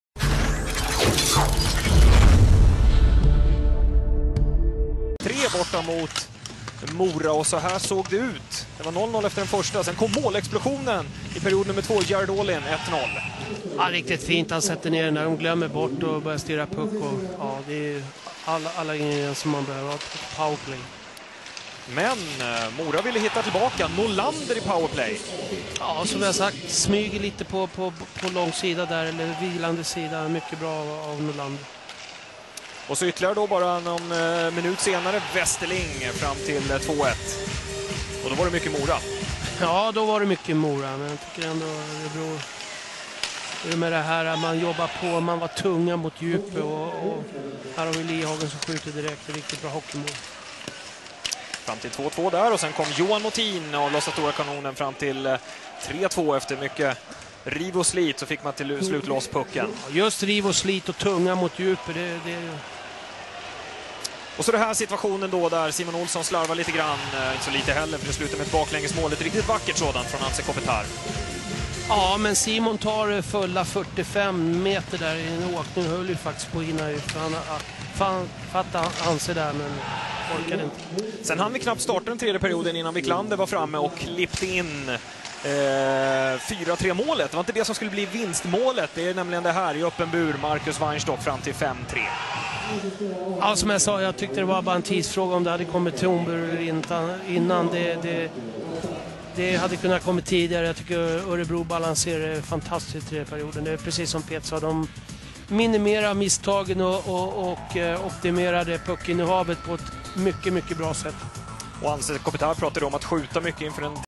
Mora-Örebro [Highlights] 3-10-12 Anze Kopitar Forsberg Goal
Anze Kopitars amazing FORSBERG goal
Hockey Allsvenskan 2012
Tabbel:
             S  V  F VÖ FÖ  GM-IM    P
  1 Örebro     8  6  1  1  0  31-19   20
  2 Troja/Lju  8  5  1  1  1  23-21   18
  3 Leksand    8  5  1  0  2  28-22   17
  4 Karlskoga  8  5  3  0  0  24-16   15
  5 Oskarsham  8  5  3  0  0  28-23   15
  6 Västerås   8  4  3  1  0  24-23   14
  7 Tingsryd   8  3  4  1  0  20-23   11
  8 Mora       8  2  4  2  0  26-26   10
  9 Malmö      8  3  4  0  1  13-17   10
 10 Djurgårde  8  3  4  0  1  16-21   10
 11 Asplöven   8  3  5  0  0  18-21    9
 12 Almtuna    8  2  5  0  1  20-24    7
 13 Södertälj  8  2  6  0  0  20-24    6
 14 Karlskron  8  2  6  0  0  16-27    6

Senaste matcher:
  3/10  Mora       - Örebro      3-5
       Oskarshamn - Leksand     4-6
       Djurgården - Karlskoga   2-4
       Almtuna    - Tingsryd    2-3
       Troja/Ljun - Asplöven    5-4
       Södertälje - Karlskrona  6-2
 5/10  Örebro     - Oskarshamn
       Leksand    - Djurgården
       Almtuna    - Malmö
 6/10  Karlskoga  - Västerås
       Tingsryd   - Troja/Ljun
       Asplöven   - Södertälje
 7/10  Karlskrona - Mora
 9/10  Västerås   - Leksand
       Troja/Ljun - Almtuna
       Södertälje - Tingsryd
       Mora       - Asplöven
 10/10 Djurgården - Örebro
       Malmö      - Karlskoga
       Karlskrona - Oskarshamn